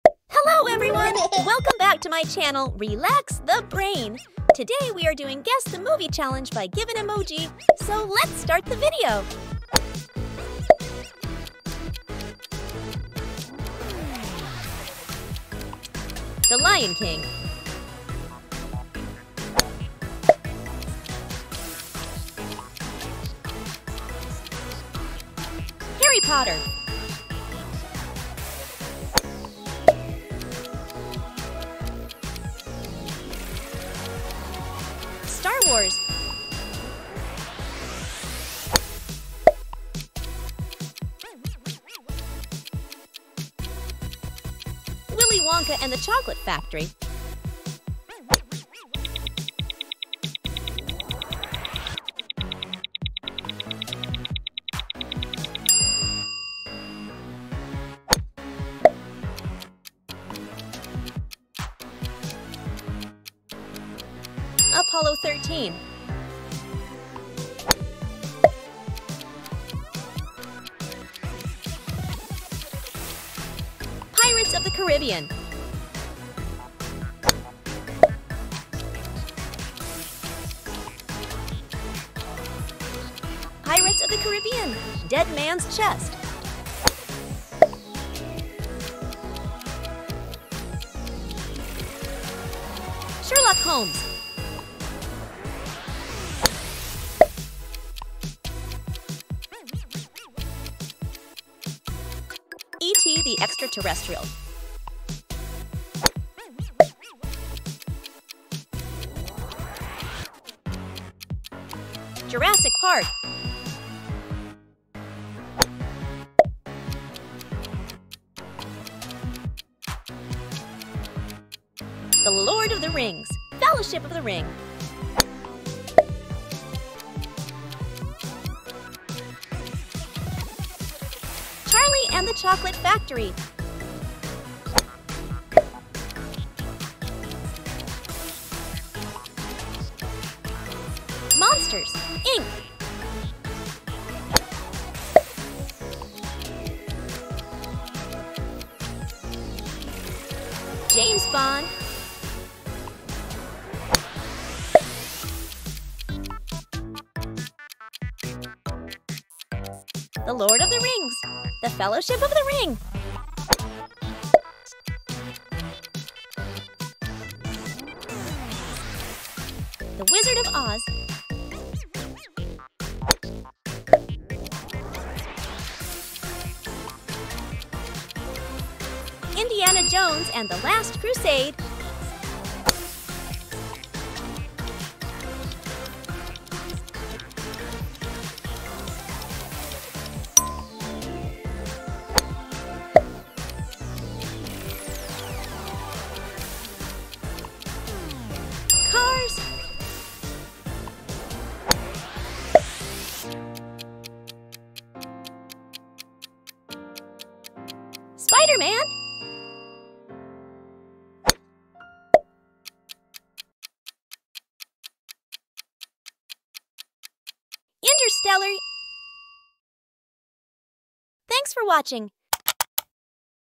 🔍 Emoji Movie Quiz: Guess the movie challenge!! 🍿🎬
🎥 Welcome to the ultimate Emoji Movie Quiz! 🤔🍿 Test your film knowledge and see if you can decipher the cinematic clues hidden in emojis! From classic blockbusters to beloved indie gems, this challenge has it all.

👀 Challenge yourself and your friends to see who can guess the movie titles first! 🕵️‍♂️🕵️‍♀️ Get ready for a fun and entertaining journey through the world of cinema, filled with laughter and surprises.

🚀 Whether you're a movie buff or just love a good challenge, this video is for you! Who will be crowned the Emoji Movie Master?